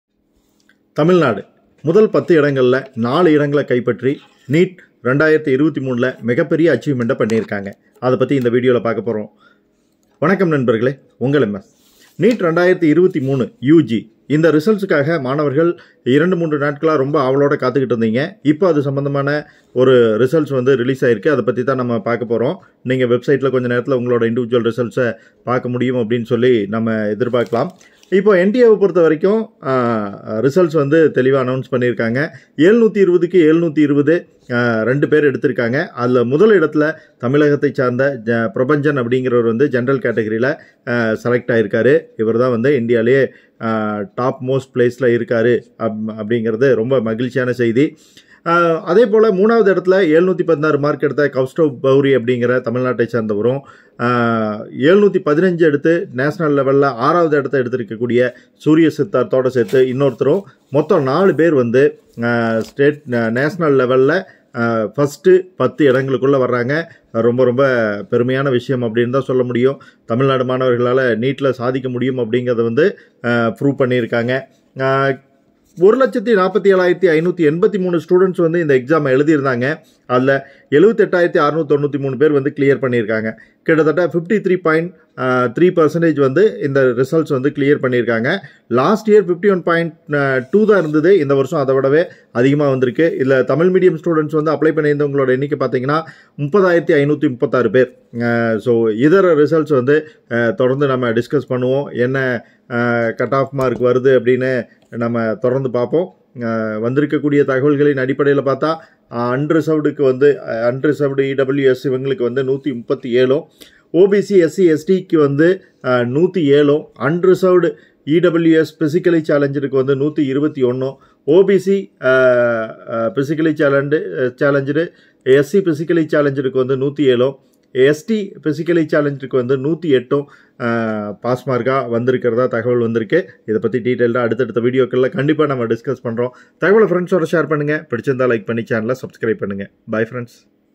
NEET UG 2023 Result Released | Tamilnadu student achieved the top rank | NEET UG 2023 Pass Mark |mse
NEET UG 2023 Result Released | Tamilnadu student achieved the top rank | mse